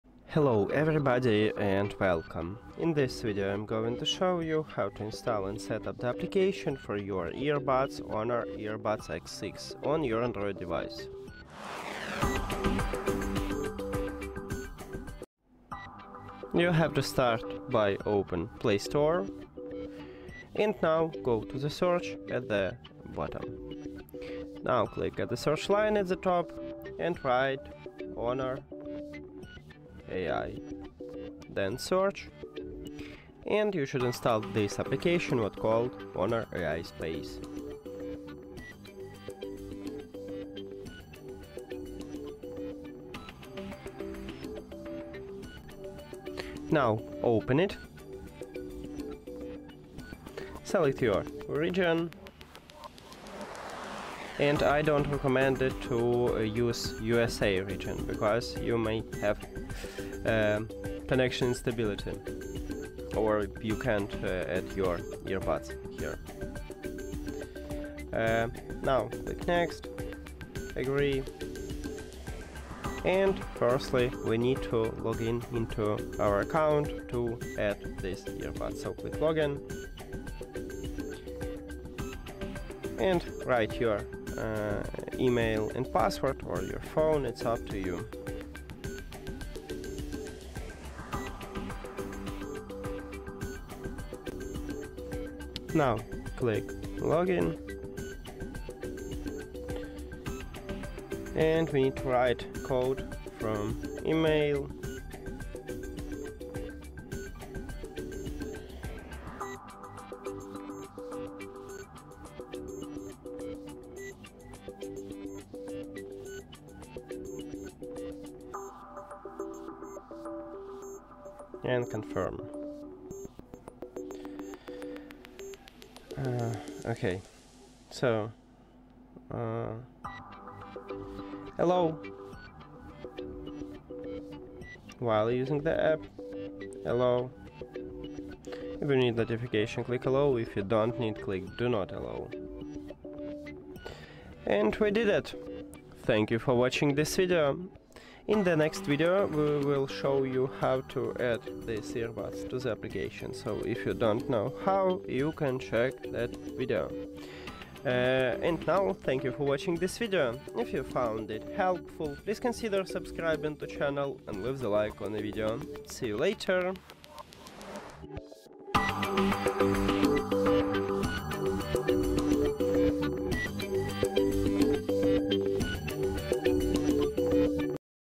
HONOR Earbuds X6 – How to Install and Set Up the App on Android
If you have HONOR Earbuds X6 and want to use all their features on your Android phone, this video will show you exactly how to install and set up the official app. We’ll guide you step by step, starting from opening the Google Play Store, searching for the correct app (HONOR AI Space), and making sure you select the right region for the best connection. You’ll also learn how to log in or create an account, and what to do if you have trouble with the setup. This process will help you connect your HONOR Earbuds X6 to your Android device and get the most out of your wireless earbuds.

How to install the HONOR Earbuds X6 app on Android?
How to set up HONOR Earbuds X6 with the HONOR AI Space app?
Why can't I add HONOR Earbuds X6 in the USA region on Android?

0:00 Introduction
0:11 Open Play Store and search for HONOR AI Space
0:24 Install and open the HONOR AI Space app
0:46 Select region and important region tips
1:08 Log in or create an account
1:35 Confirm login and set up notifications
2:23 Setup complete and next steps